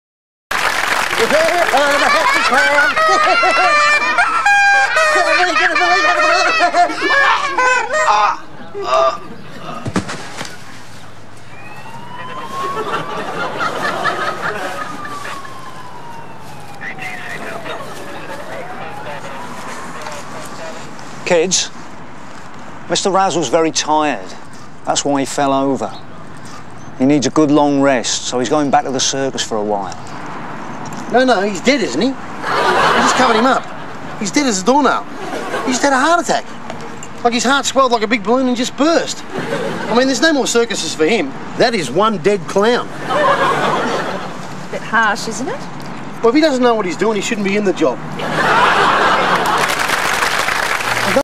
Russell Gilbert Honest Paramedic
Russell Gilbert is perhaps a touch too honest for this young crowd?
Follow Russell here: @russellgilbertcomedian on INSTAGRAM